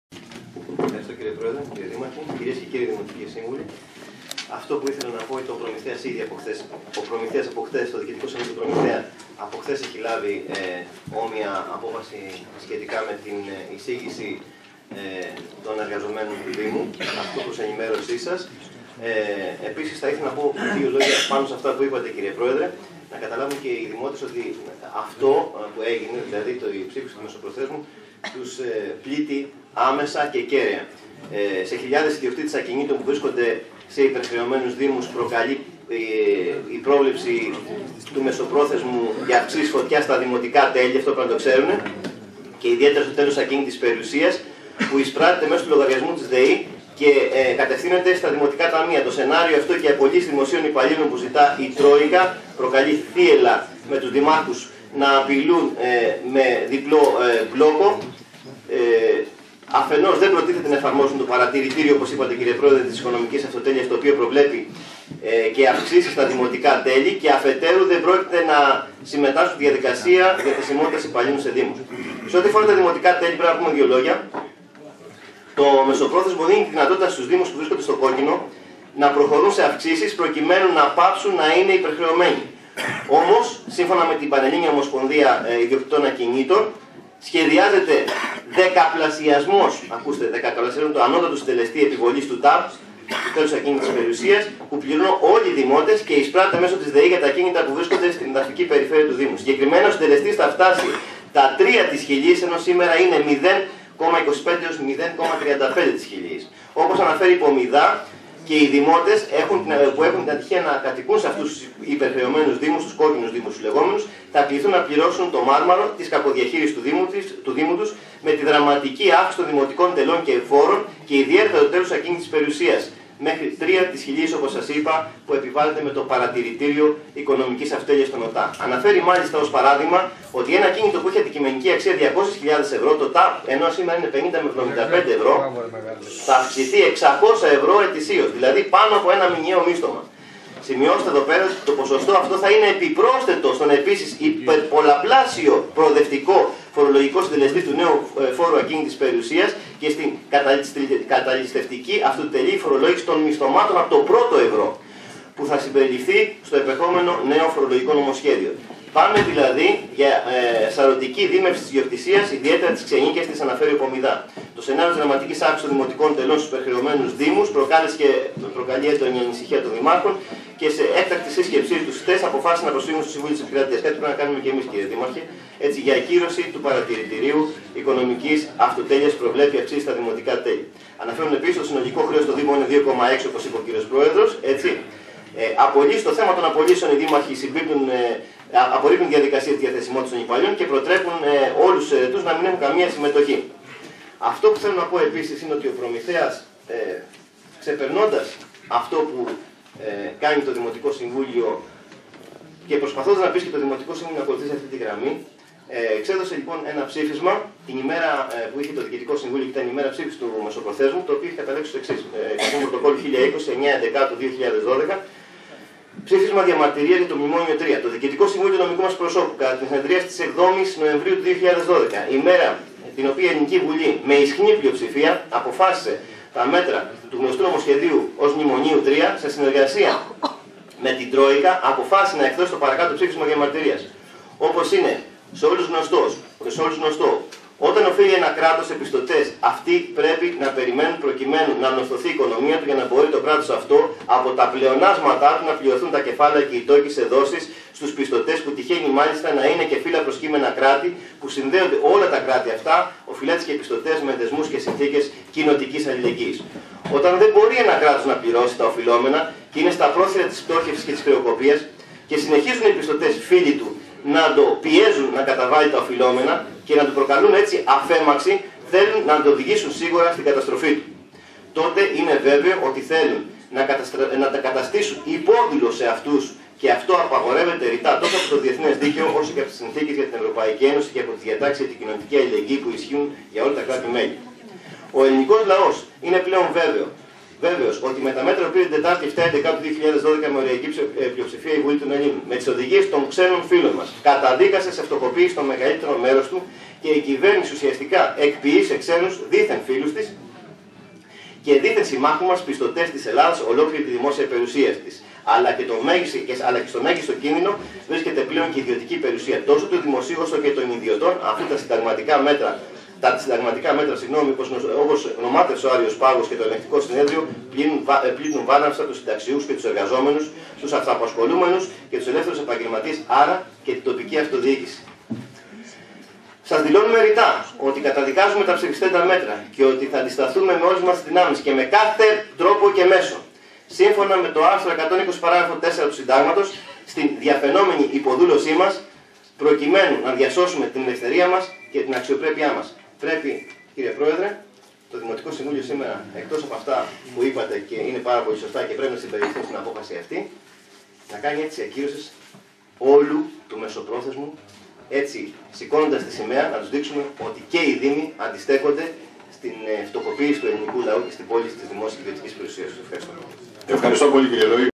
Ν. Λόης Πρόεδρος ΝΠΔΔ ΠΡΟΜΗΘΕΑΣ Έκτακτο Συμβούλιο 3
Ν. Λόης Πρόεδρος ΝΠΔΔ ΠΡΟΜΗΘΕΑΣ Έκτακτο Δημοτικό Συμβούλιο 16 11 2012, Εργαζόμενοι,επιπτώσεις στην Τοπική Αυτοδιοίκηση μετά  την ψήφιση του ν. 4093 κάθετος 2012, μεσοπρόθεσμο,tvtv,Δήμος Λουτρακίου Αγ Θεοδώρων μέρος 3.mp4